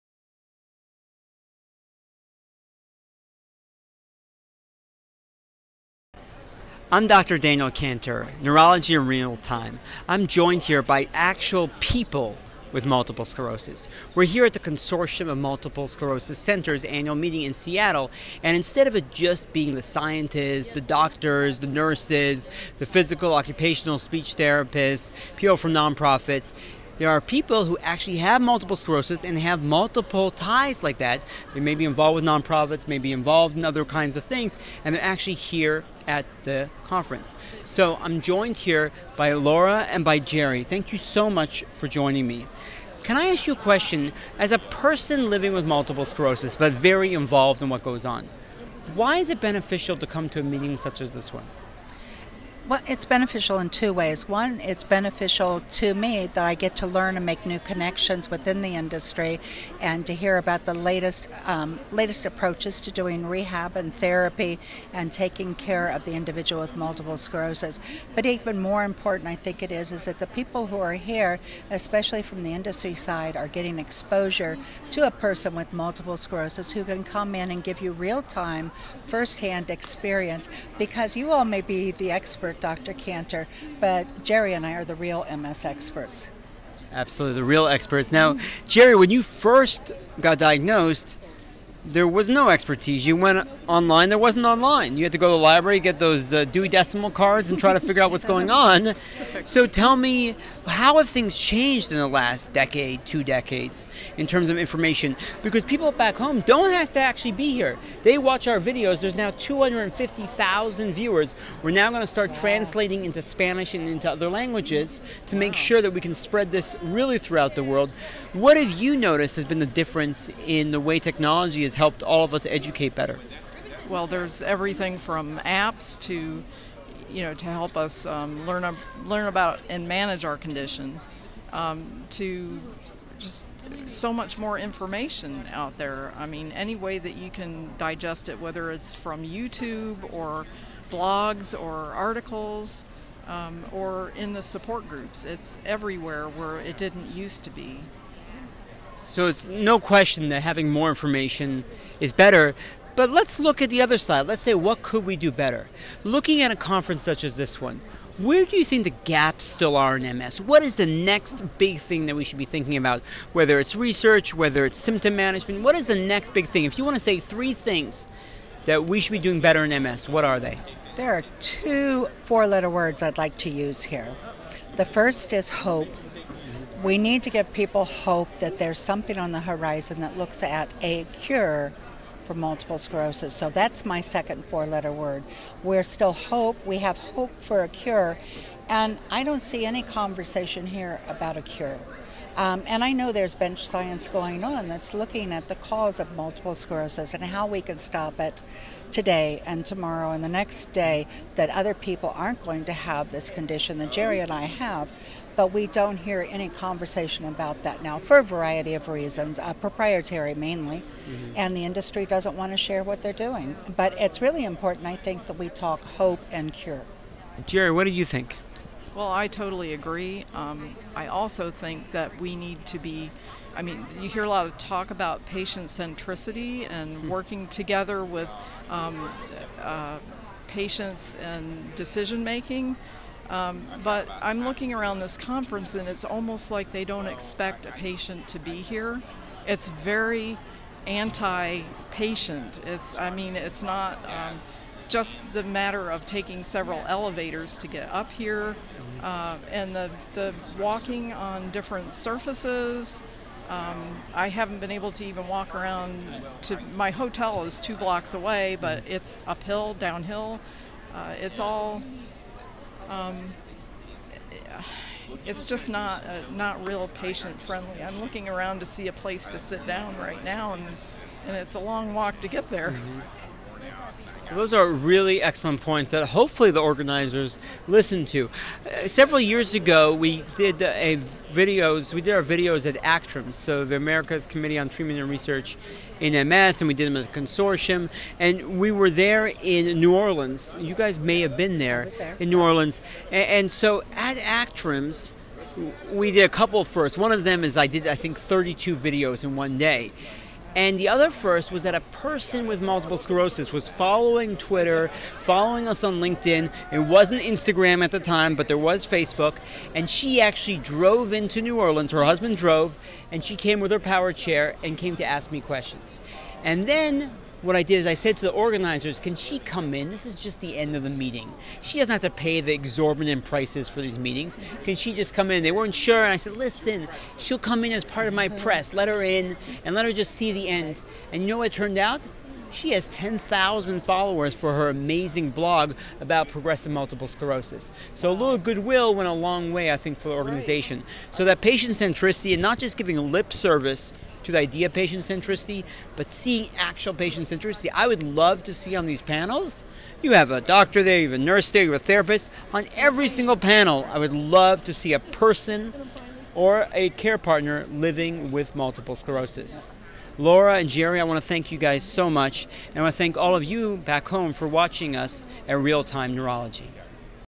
LDN (low dose nalrexone)
Real Time Neurology's Daniel Kantor, MD interviews Gary Tomas, MD about LDN (low dose naltrexone) for MS.